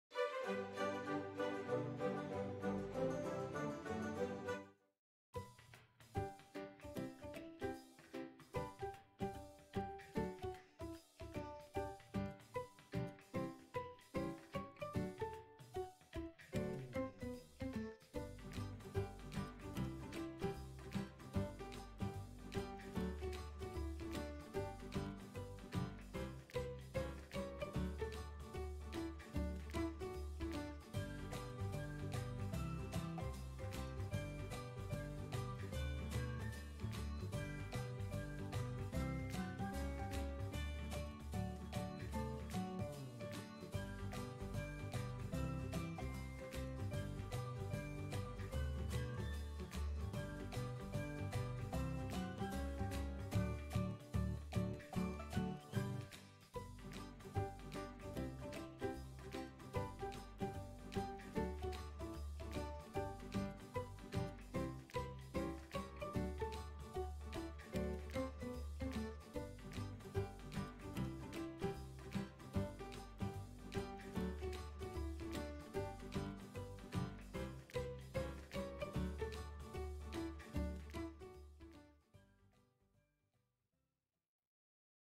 Writing a Pangram in English Cursive using Guidelines - Pelikan M1000 Black - Handwriting practice
Pen: Pelikan M1000 Black
Nib: Pelikan18k Gold n.8 Fine
Ink: PenVenture Black